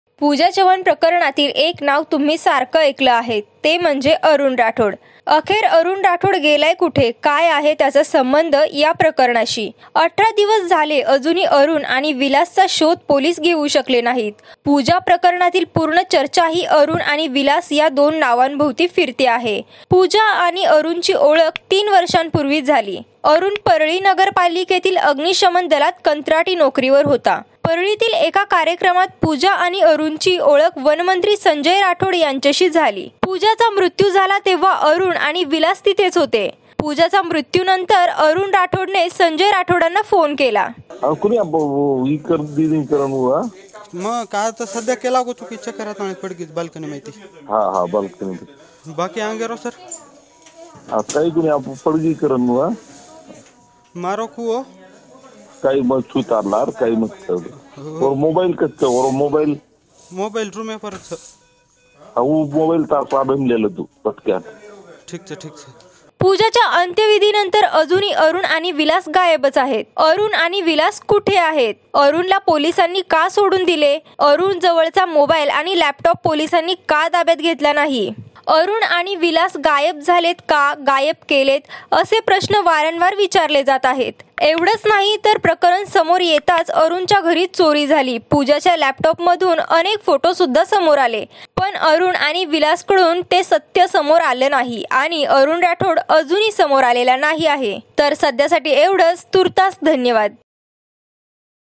पूजा जोडीला यांची पहिली भेट झाली कुठे ? | पूजाला ३ वर्षा पासून ओळखायचा तरीही राठोडांचा खास अरुण !
पूजा चव्हाण आत्महत्या प्रकरण महाराष्ट्रभर गाजतंय. शिवसेनेचे मंत्री संजय राठोड यांना या प्रकरणी लक्ष्य करत राजीनाम्याची मागणी जोर धरतेय. दुसरीकडे काही व्हायरल ऑडिओ आणि फोटो समोर येत आहेत. हे सगळं एका बाजूला असताना मूळ पूजाकडे काही दुर्लक्ष होतंय. या संबंधीचा हा स्पेशल रिपोर्ट. टिक टॉक वर पूजा प्रसिद्ध होतीच पण फेसबुकवर पूजा स्वतःचा फॅन क्लब वाढवू इच्छित असल्याचं दिसून आलंय.  पूजाचे फक्त स्वतःचे अकाउंट नाही तर तिच्या समर्थनार्थ अनेक अकाउंट्स आहेत. अगदी मराठवाड्यातल्या सगळ्या जिल्ह्यांचा यात उल्लेख आहे. आणि त्यामुळेच तिची राजकीय महत्वाकांक्षा लपत नाही. पूजाताई चव्हाण नावाने बीड, औरंगबाद, परभणी, लातूर  असे अकाउंट्स आहेत. या अकाउंटला शेकडो फ्रेंड्स असून फॉलोवर्सची संख्याही हजारात आहे. या अकाउंटवर पूजाला अनेकांनी वाढदिवसाच्या शुभेच्छा दिल्यात पण त्याही नेत्यांना दिल्या जातात तशा. थोडक्यात नेतृत्व करण्याची पूजाची इच्छा यातून दिसून येते.